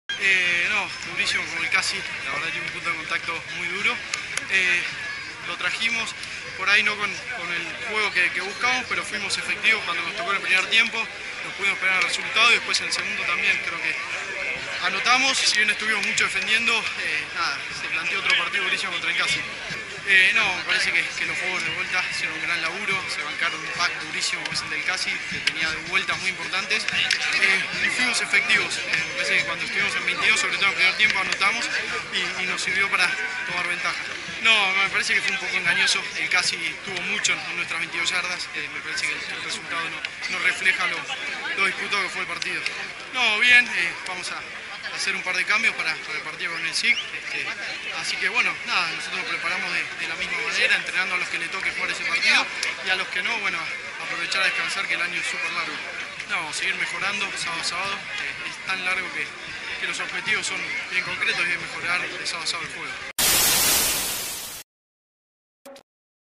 Joaquín Hardoy - CUBA
El centro cubano fue el jugador más destacado en el clásico ante CASI.